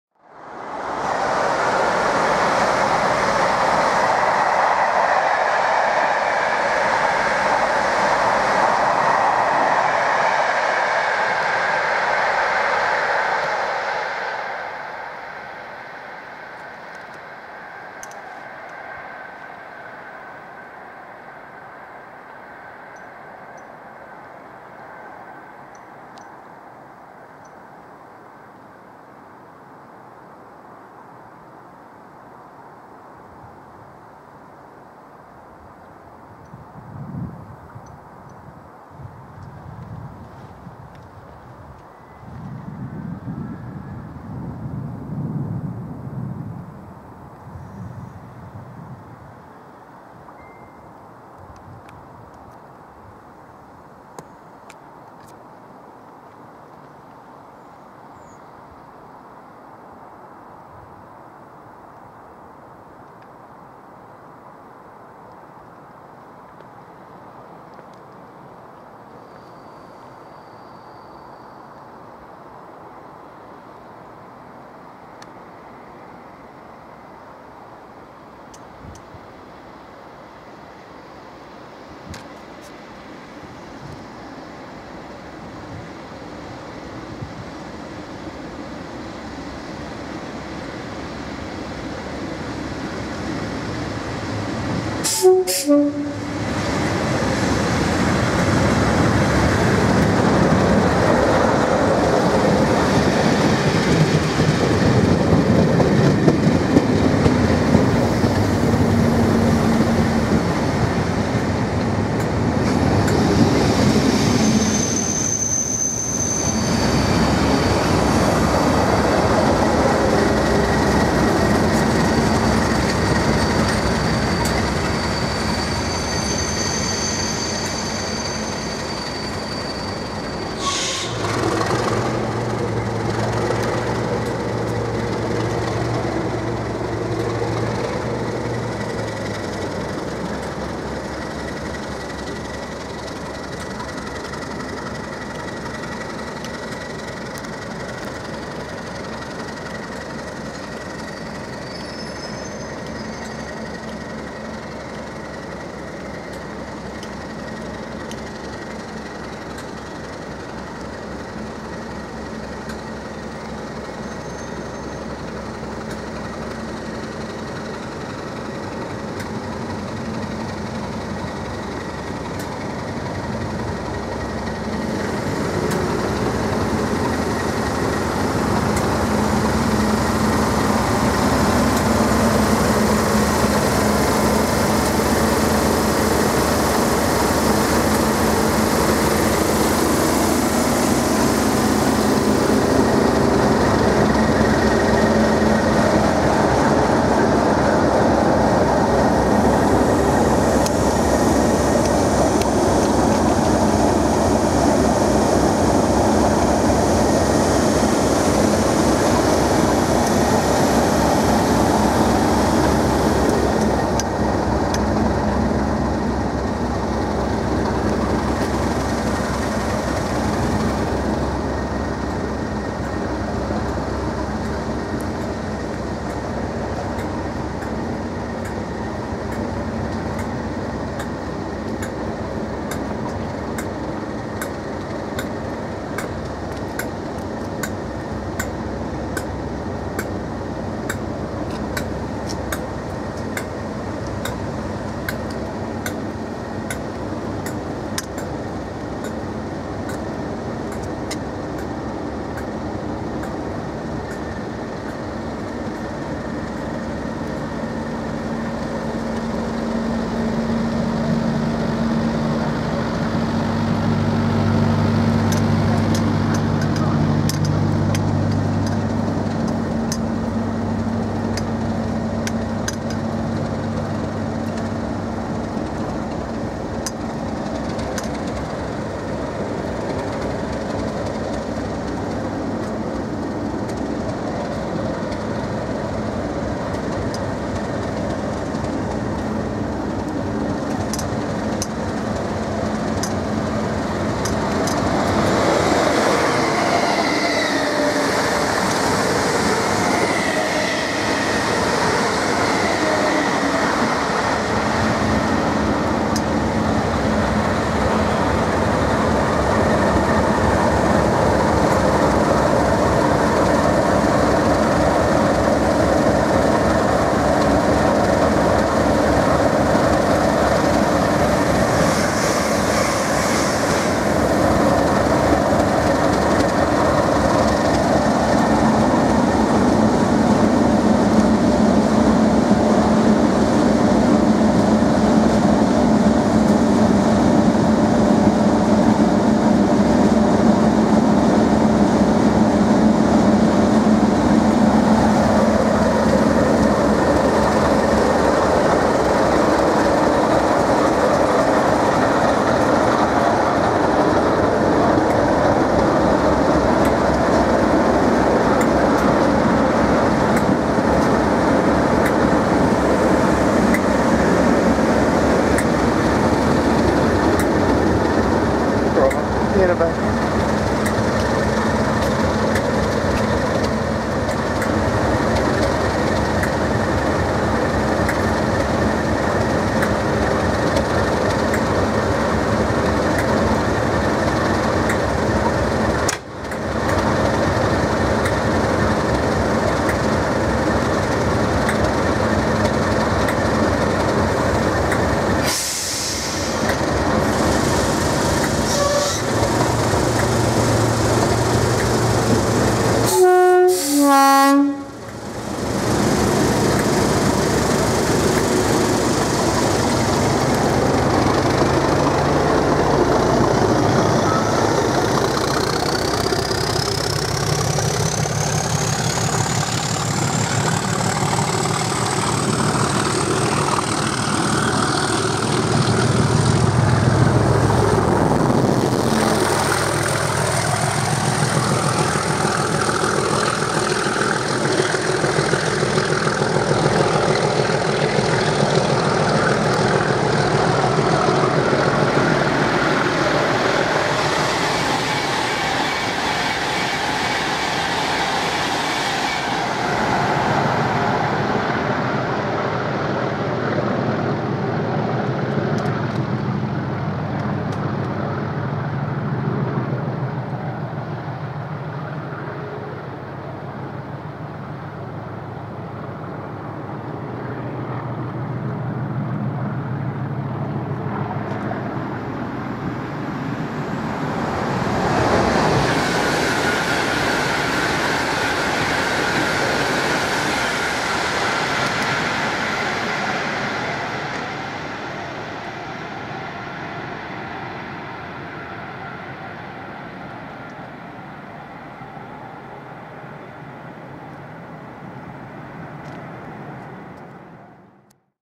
Colas operated, 37601 tnt 37604 on Test Train arrive and depart Tring with noise - 7th December 2016
Here,  DRS 37/6's (601 tnt 604) operating 1Q11 Derby RTC to Old Oak Common H.S.T.D. arrive and depart Tring for their booked stop, with a very friendly Colas driver providing rather large noise and clag.